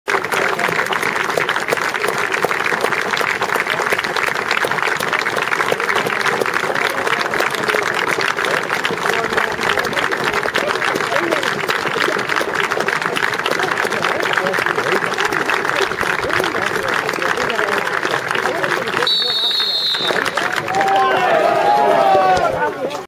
The Clitheroe fc legends.MP4
Players turn out for Keith Lord at Clitheroefc.